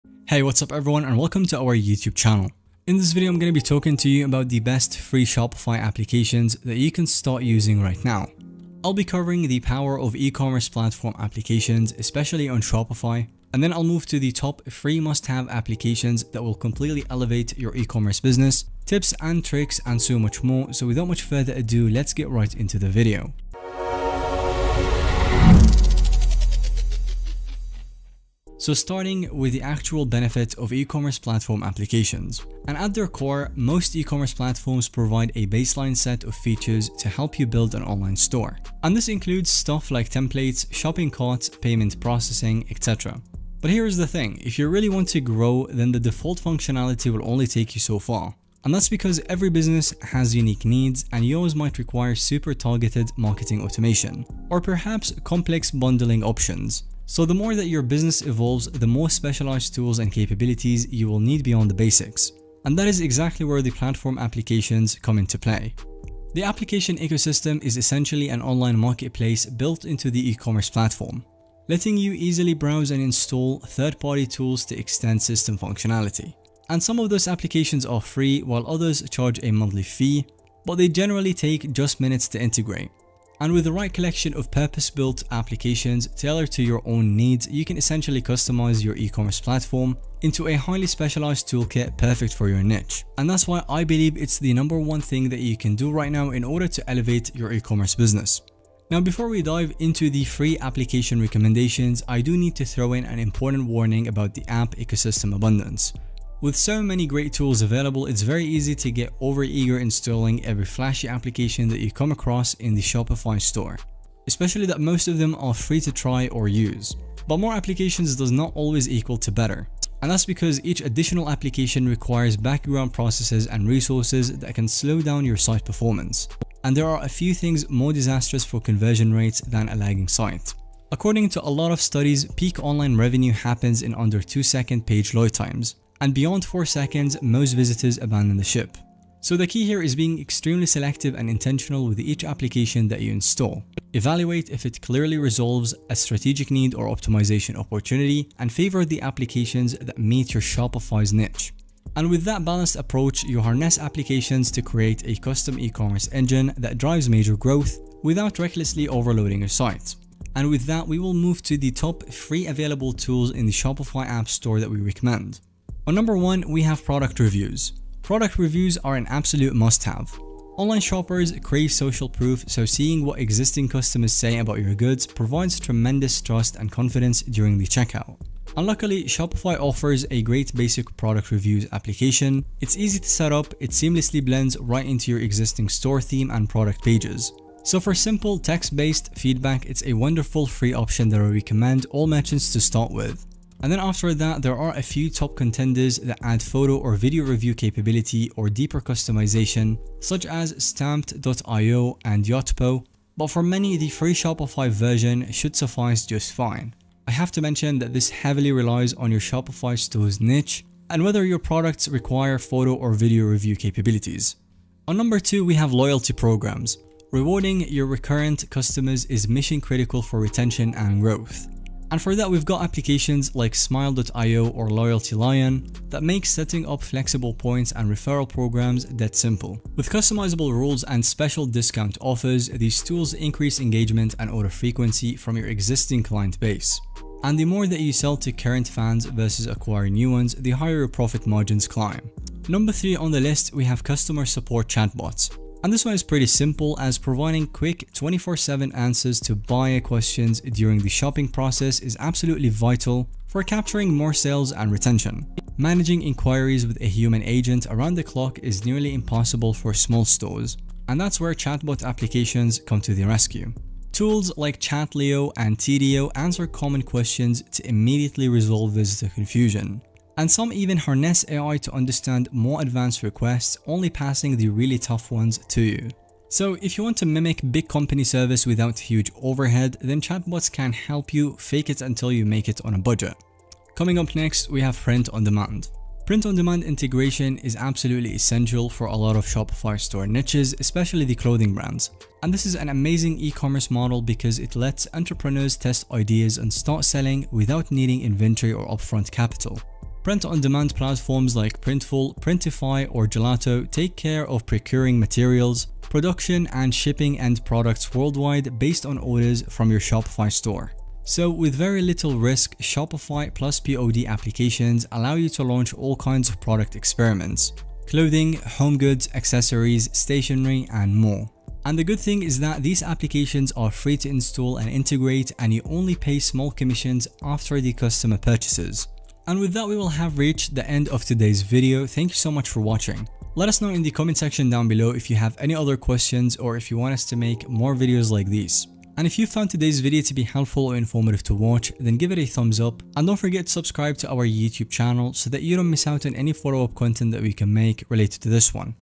Best FREE Shopify Apps In 2025 | Optimize Your Shopify Store!
✅ Open your Shopify store for only $1/month (Temporary offer)

🚀 Boost your Shopify store without breaking the bank! 🛒💡 We've handpicked top-notch apps that won't cost you a dime, helping you enhance your store's functionality, user experience, and overall performance. 🌐 From marketing to customer support, we've got you covered with practical recommendations that can make a significant impact on your business. 💰 Don't miss out on these valuable tools – watch now and level up your Shopify game! ⭐🔧

⏱️ Timestamps
Overview 00:00
The True Power of Ecommerce Platform Apps 00:30
Beware the App Overload Trap! 01:36
#1 Product Reviews 02:40
#2 Loyalty Programs 03:35
#3 Customer Support Chatbots 04:05
#4 Print on Demand 04:40